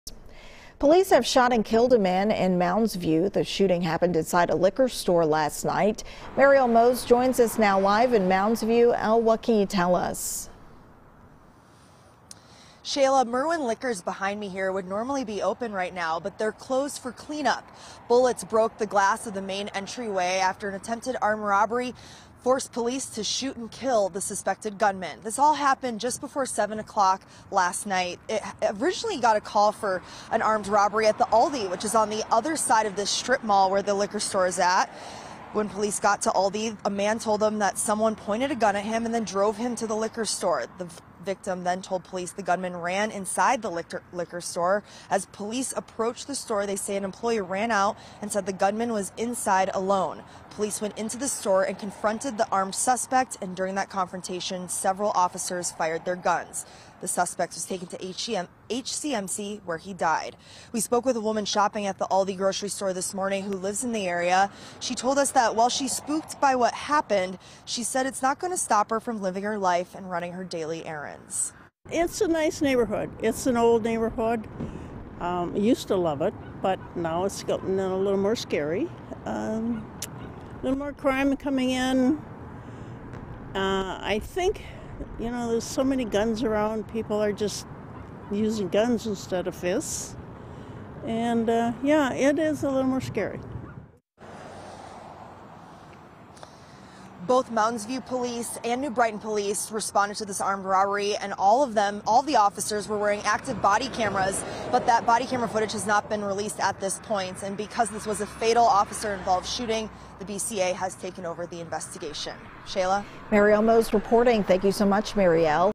Police Fatally Shoot ‘Armed’ Suspect At Mounds View Liquor Store
Police shot and killed a man who they say had a gun in a liquor store in Mounds View Sunday evening.